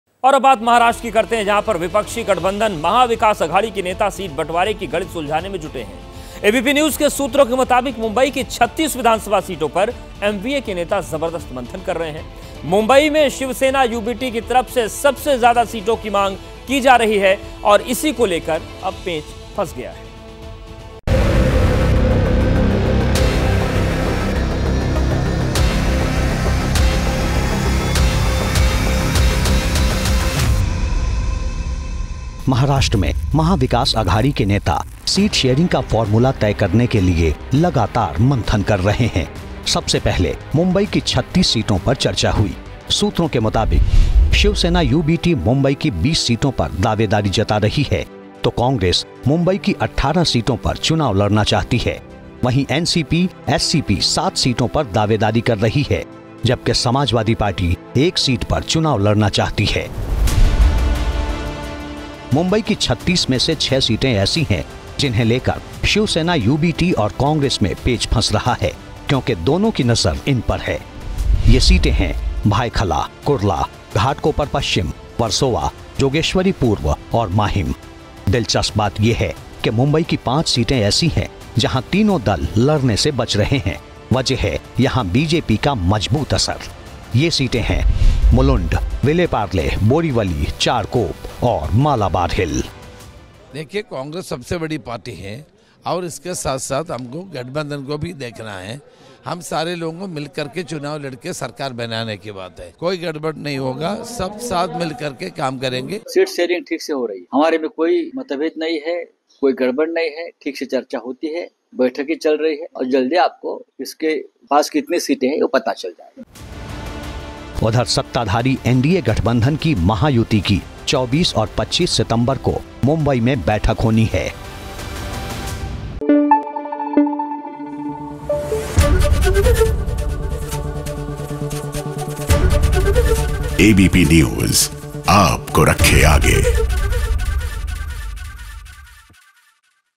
Election 2024: शिवसेना(UBT) की तरफ से की जा रही है सबसे जयादा सीटों की मांग | ABP LIVE Mumbai
Election 2024: MVA में चल रहा सीटों का मंथन, शिवसेना(UBT) की तरफ से की जा रही है सबसे जयादा सीटों की मांग..